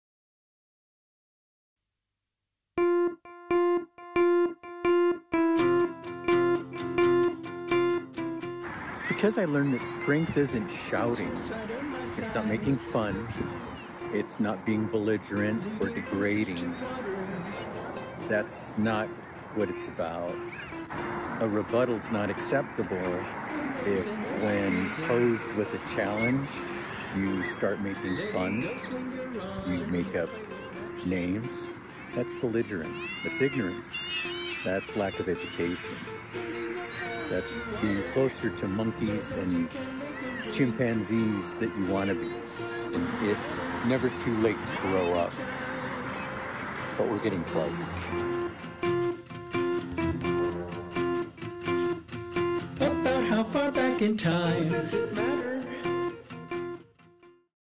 Be Nice- Indie Music Producers POV
A short video of my POV in regards to the Decency of Humanity or what it could be...Support indie music and Art, Subscribe, Like, Comment and Share this and all my videos.